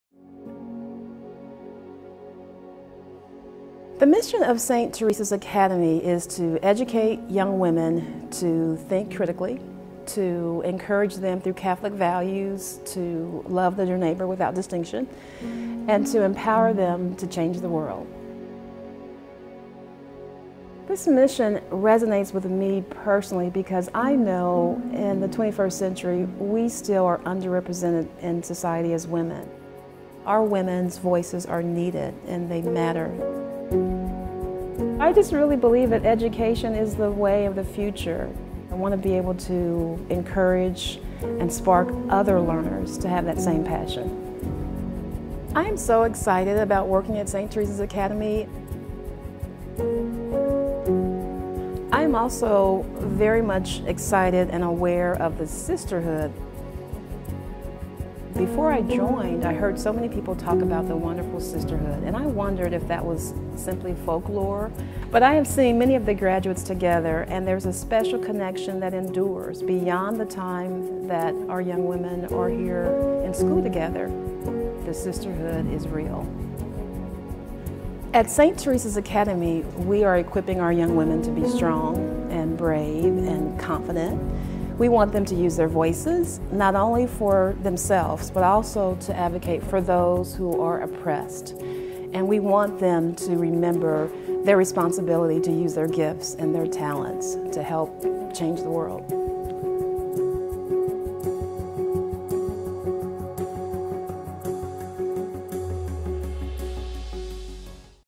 Welcome video from Dr. Siabhan May-Washington
Welcome message from St. Teresa's Academy President, Dr. May-Washington.